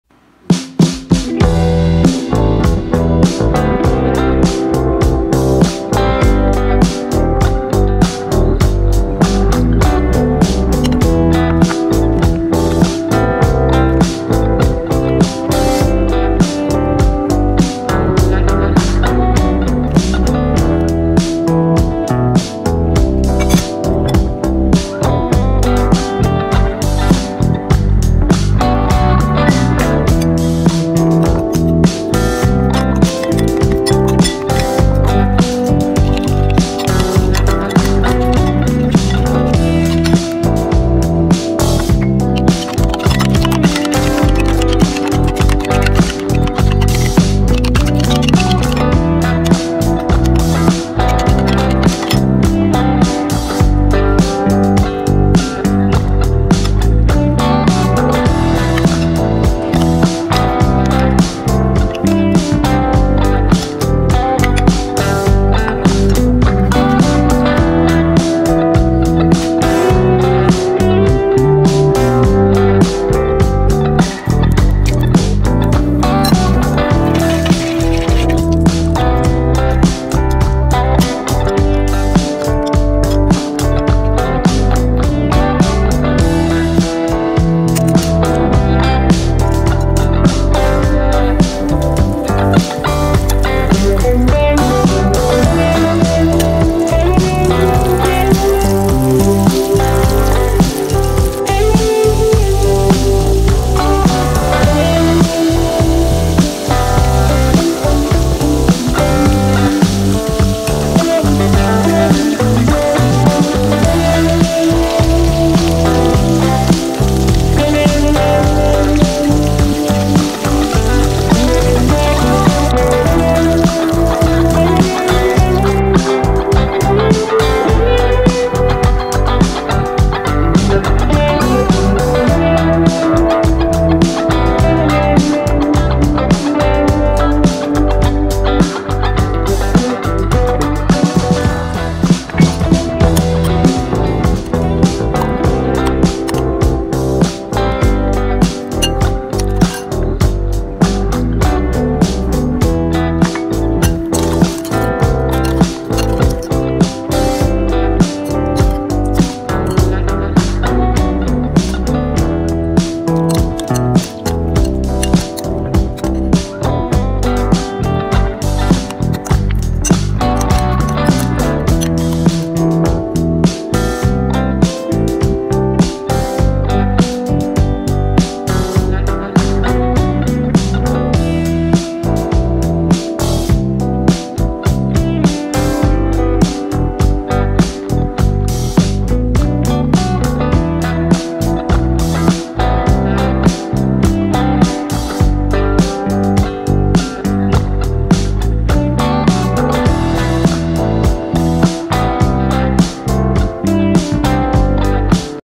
How to make​​​ crispy pork Ribs| Cambodia recipe show
Hello guys, Today i would like to show all of you about How to make​​​ crispy pork Ribs . watch and try it at home together.